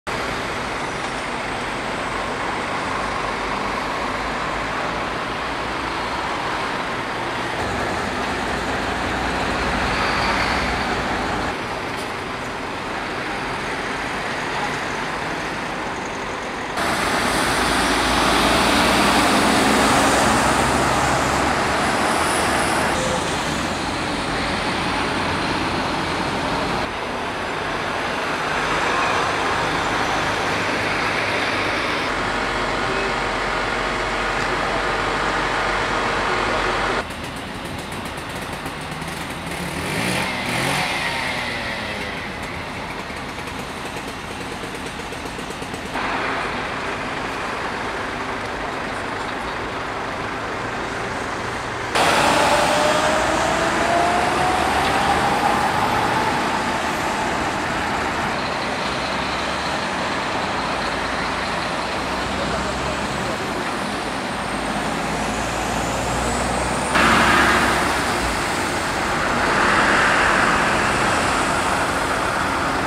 TP: Vážná nehoda tří kamionů na Pražském okruhu u Dolních Břežan
Článek: 1url.cz/stddA